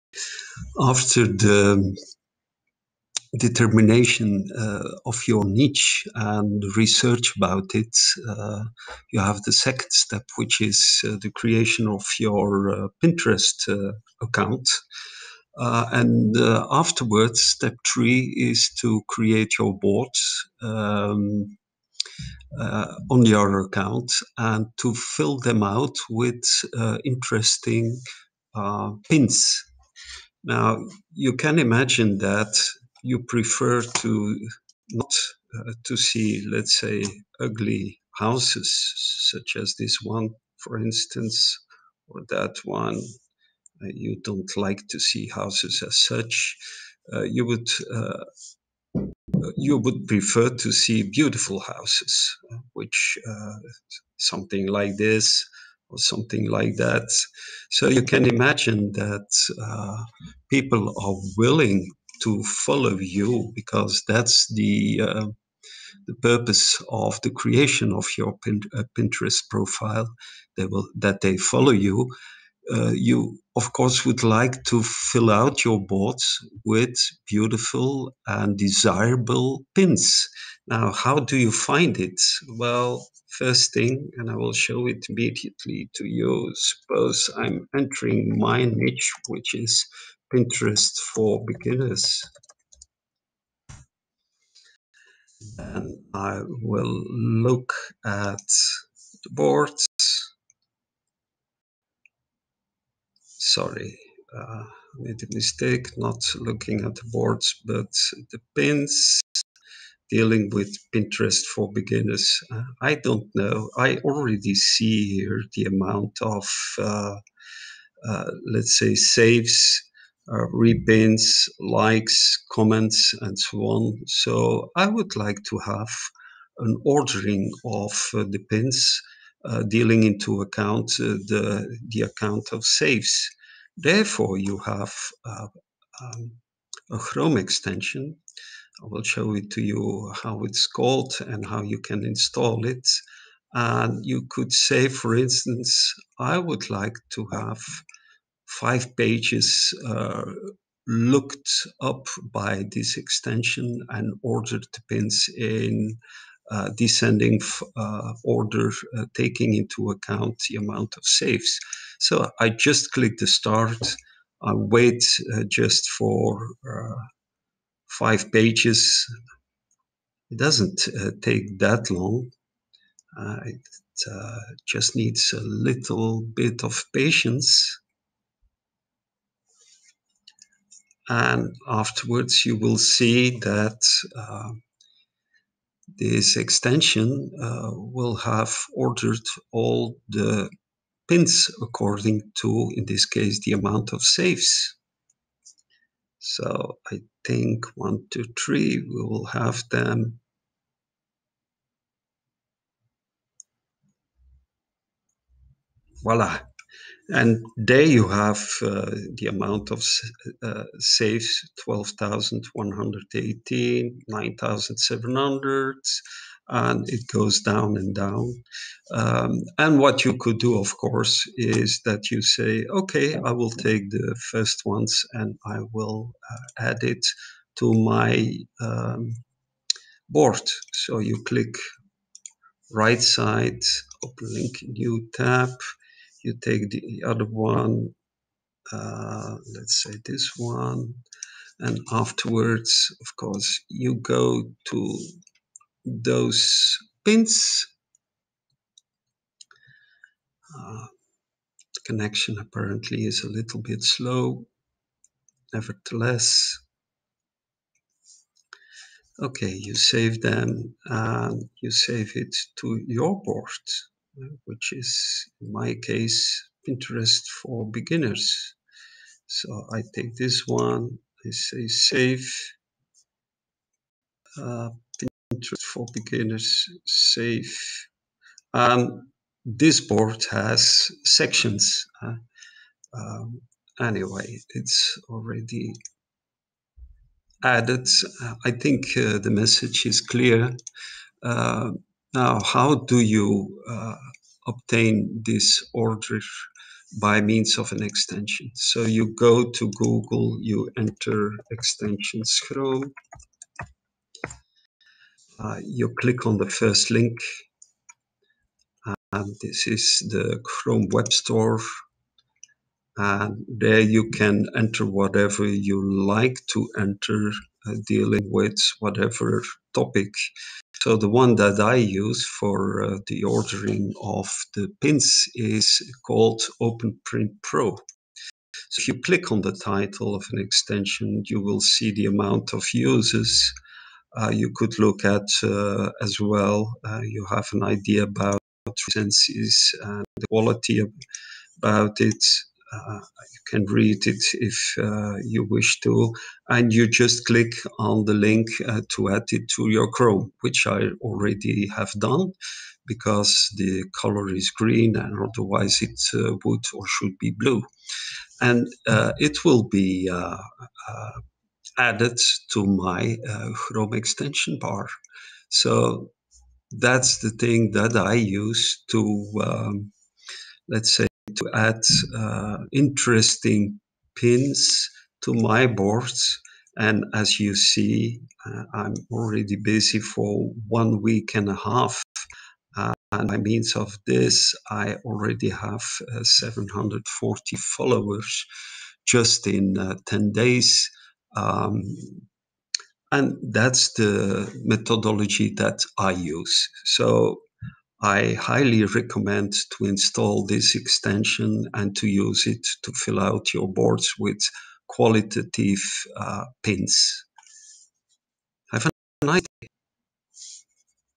Pinterest For Beginners - How to Fill Up your Boards with Qualitative Pins
Pinterest for Beginners - how to find qualitative pins to fill up your boards so that people are more willing to follow you.